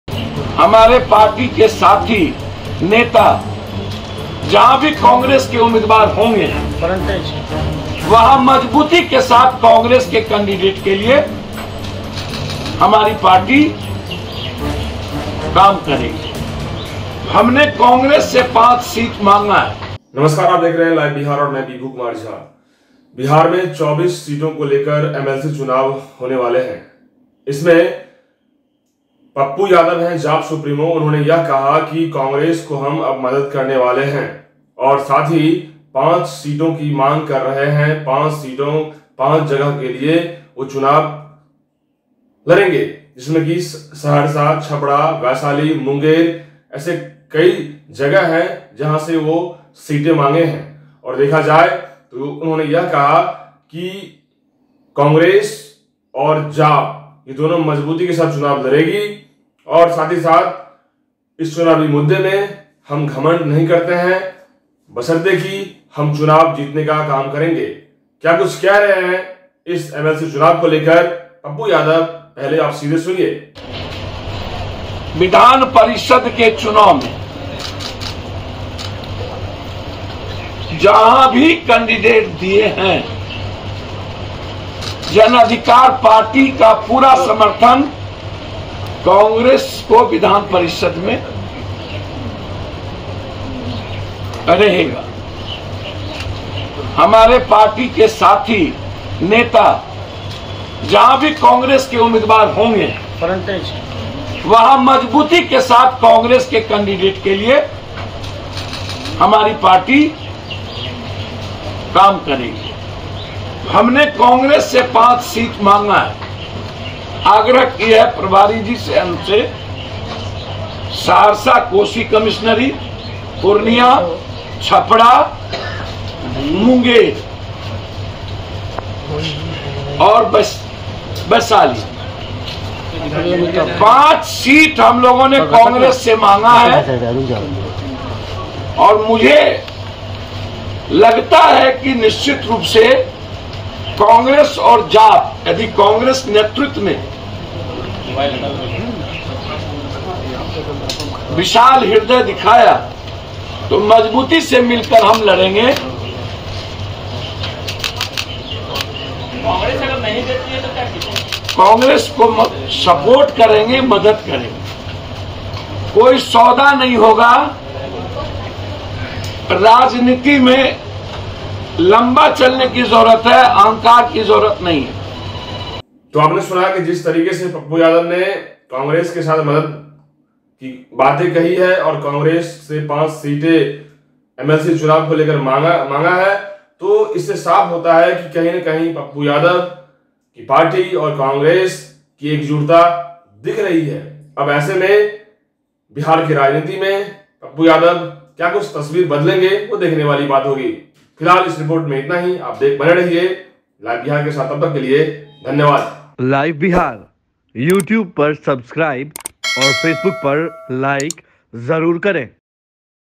MLC चुनाव में पप्पू यादव ने कांग्रेस से क्या शर्त रखी ? Live Bihar
Live Bihar Digital Network
MLC चुनाव में पप्पू यादव ने कांग्रेस से क्या शर्त रखी ?